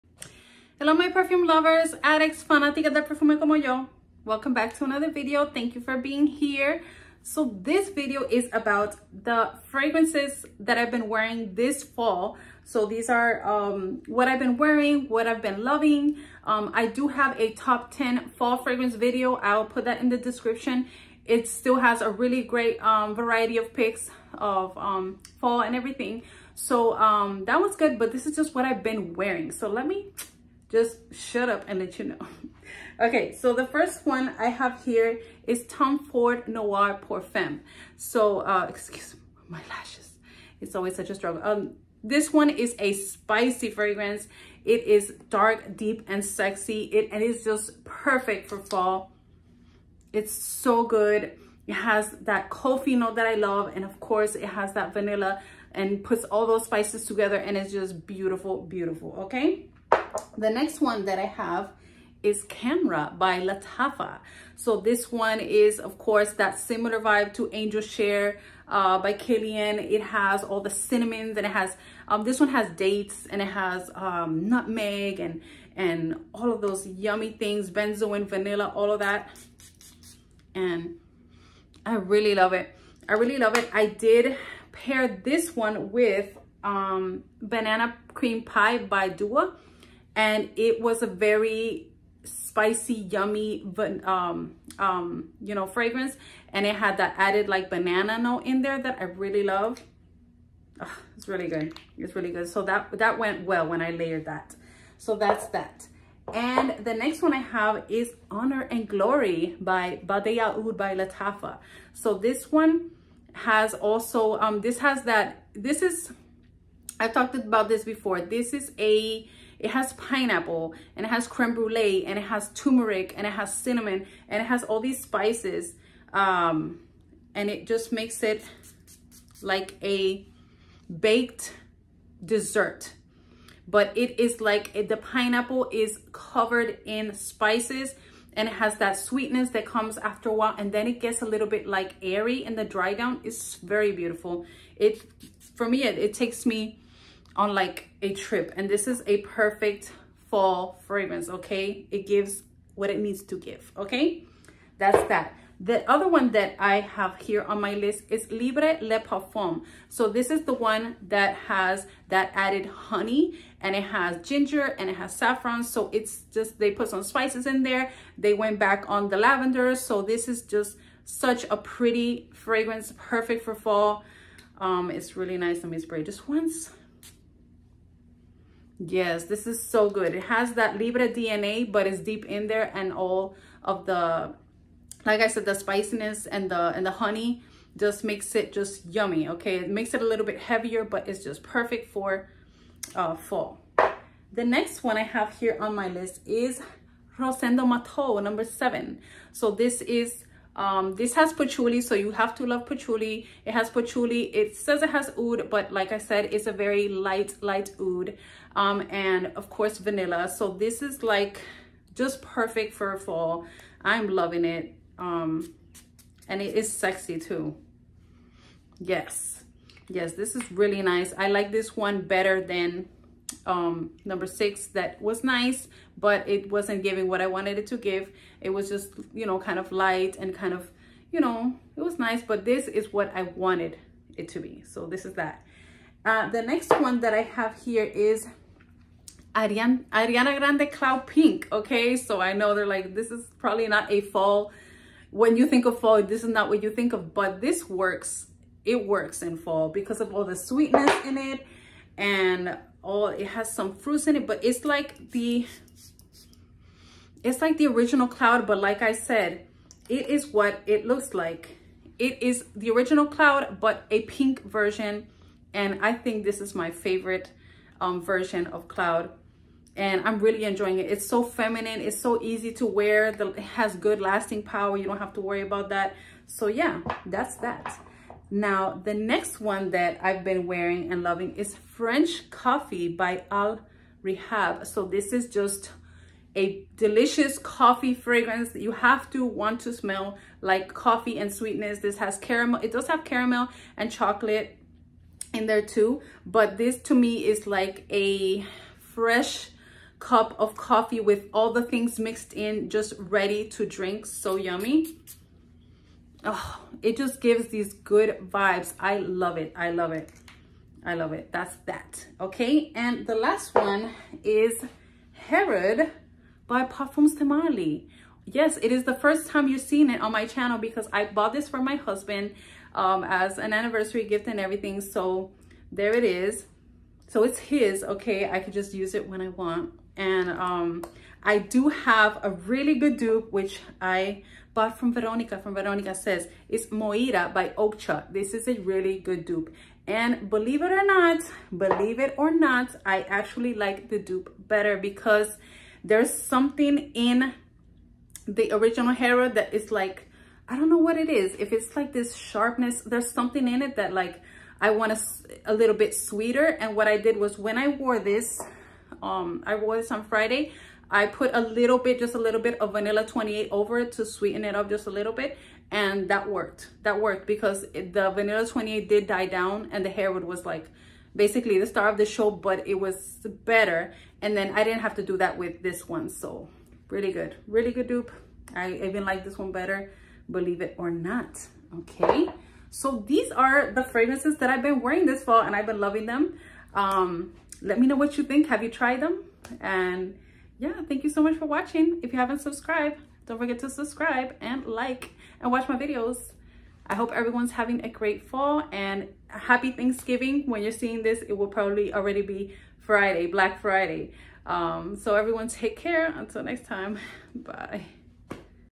Fragrances I’ve been wearing & loving this Fall 🍁💛🧡🤎❤️🍁
Hello my perfume lovers, addicts, fanaticas del perfume Como yo!

Today I am sharing with you the fragrances I’ve been wearing and craving this fall.

Take care and be blessed 😇